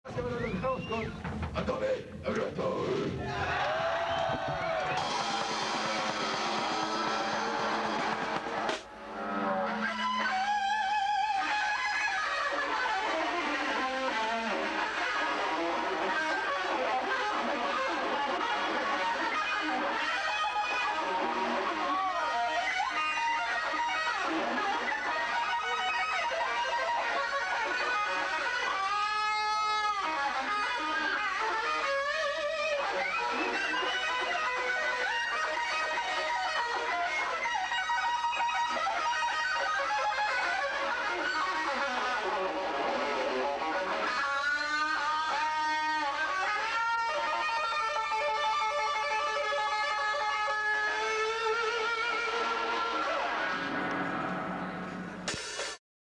Atomic Aggressor - Jaime Moya Zuñiga - Arica 1991
Sadism da el pase a Atomic Aggressor y Jaime Moya Zuñiga ( q.e.p.d. ) de atomic aggressor empieza con un solo ...